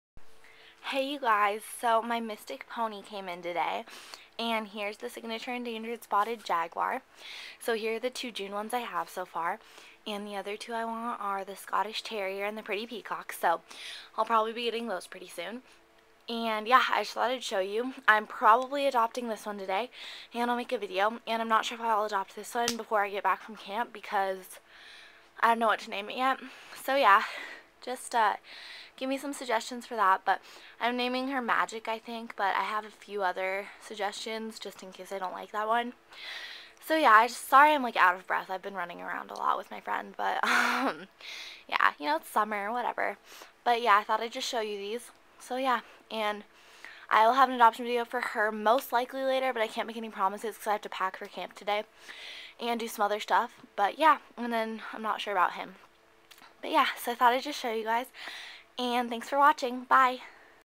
New June Pets!
I know I'm a little late haha. Please leave me name suggestions for my spotted jaguar he's gonna be a boy! Thanks!